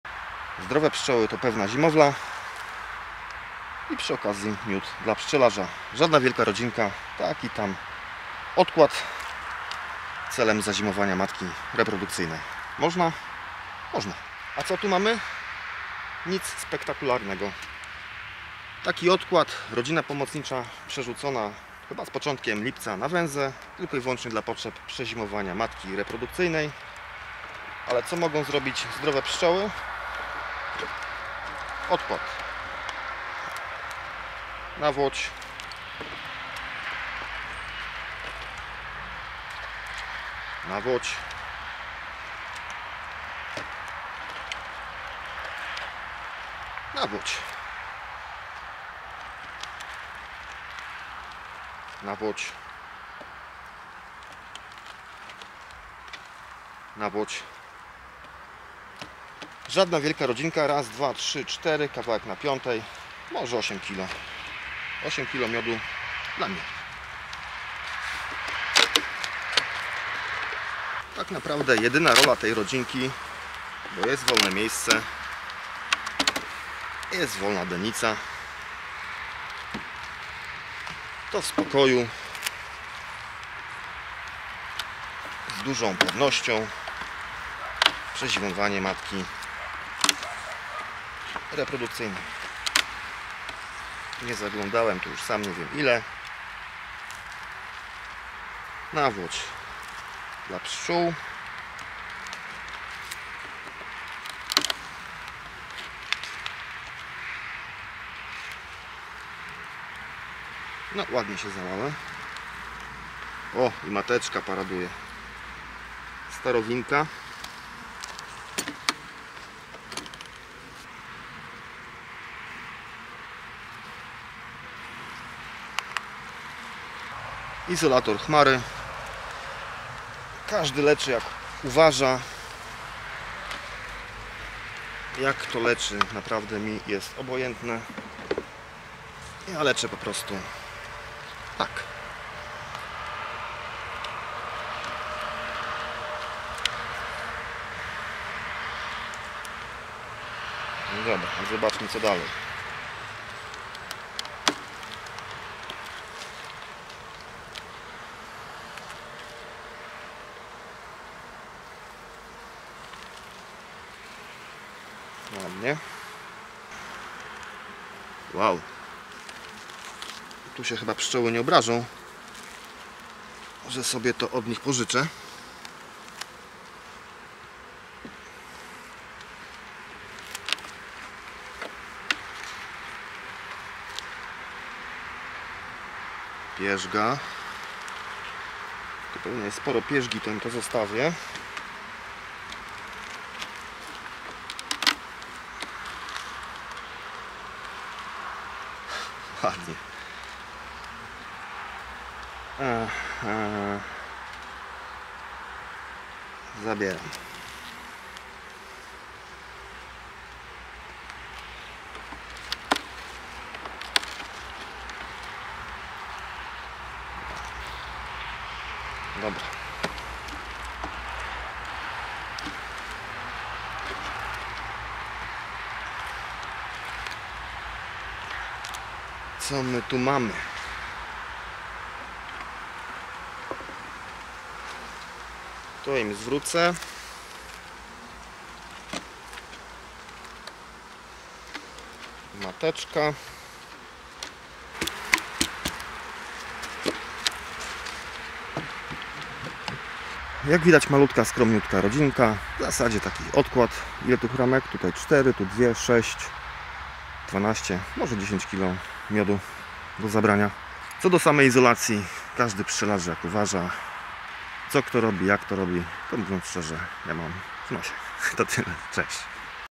Pszczoły z wigorem- czyste gniazdo i brak warozy...
Stosunkowo mała rodzina pszczela a miodu natargała jak wściekła.
Czemu? Nie tylko siła lecz i wigor rodziny pszczelej się liczy,
a ten wprost proporcjonalny do czystego gniazda i zdrowych pszczół :)
Ten odkład pszczeli jakoś w Lipcu (?) po uprzednim odebraniu wszystkich ramek z ula- pierzgi, miodu oraz czerwiu- został przesiedlony na węzę.
Pszczoły uwolnione od dręcza, na czystych plastrach wyczyniają cuda.